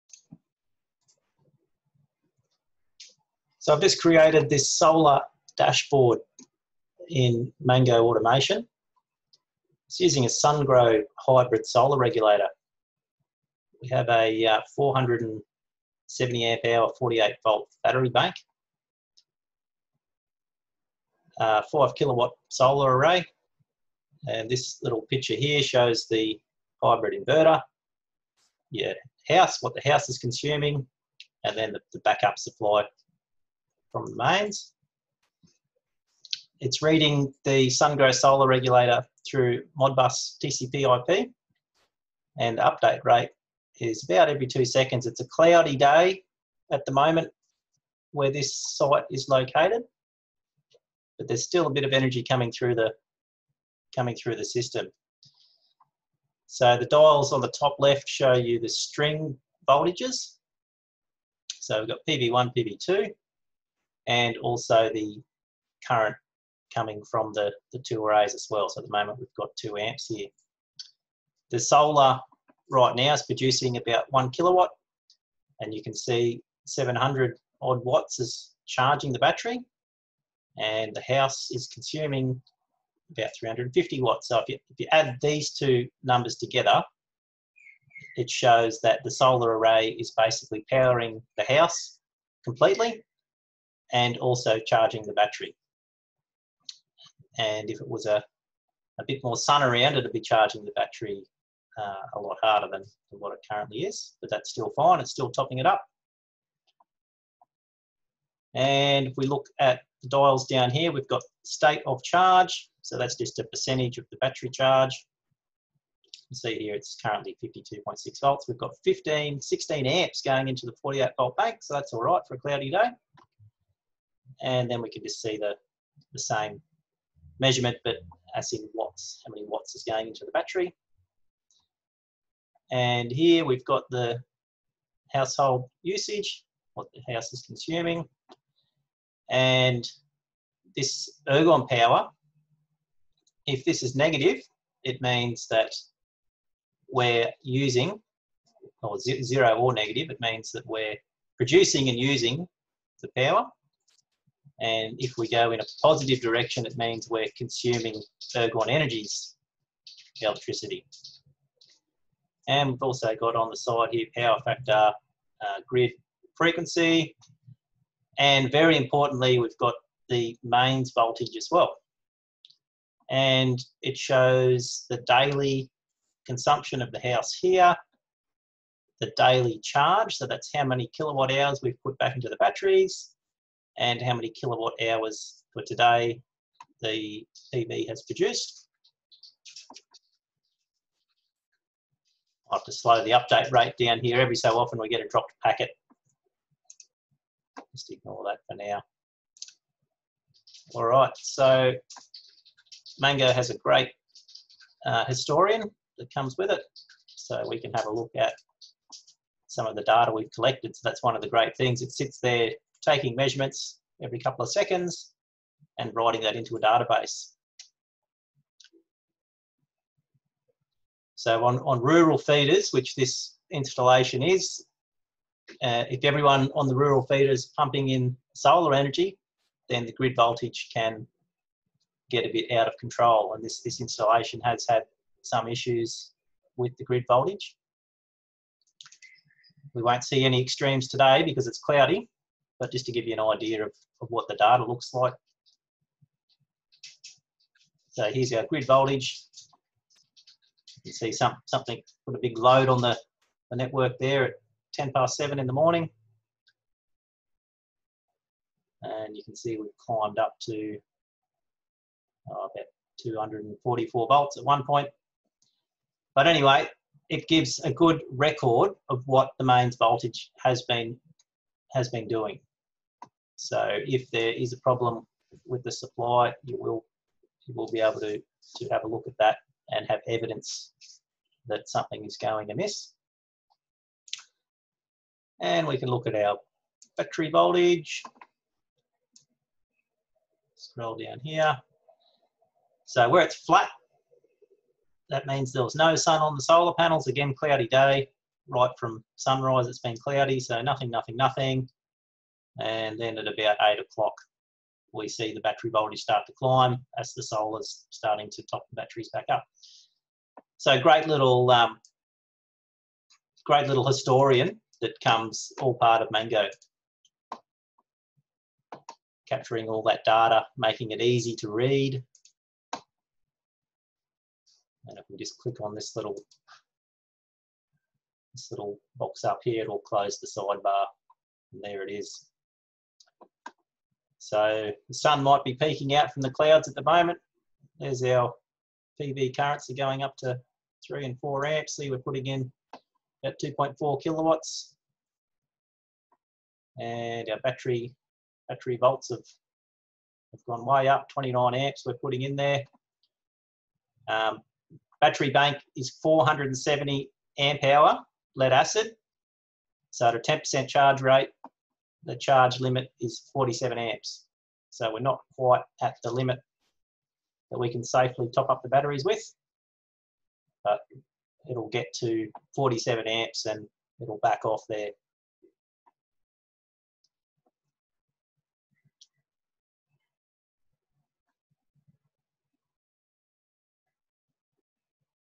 Mango Automation Hybrid PV Dashboard
Using Mango Automation to read a Sungrow hybrid PV system. Battery bank is a 48v 470Ah lead acid system.